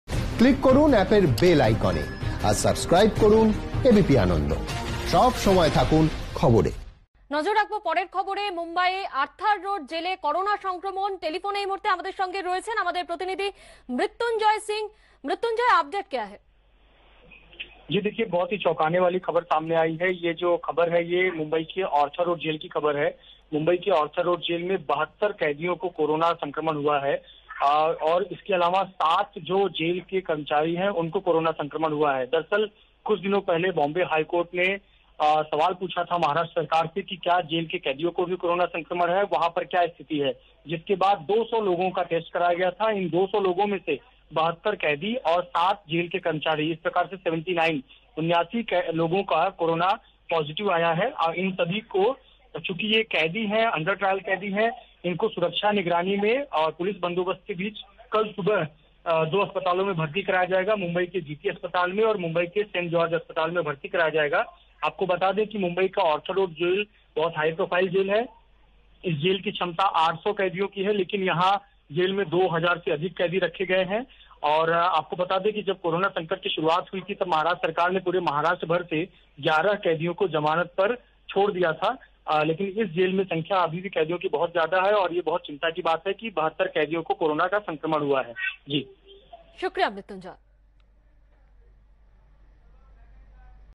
মুম্বইয়ের আর্থার রোড জেলে করোনা সংক্রমণ, আক্রান্ত ৭২ বন্দি, ৭ কর্মী
মুম্বইয়ের আর্থার রোড জেলে করোনা সংক্রমণ। আক্রান্ত ৭২ জন বন্দি। করোনা আক্রান্ত জেলের ৭ জন কর্মীও। মুম্বইয়ের জিটি হাসপাতালে ভর্তি করা হয়েছে আক্রান্তদের। মহারাষ্ট্র জেল কর্তৃপক্ষ সূত্রে খবর।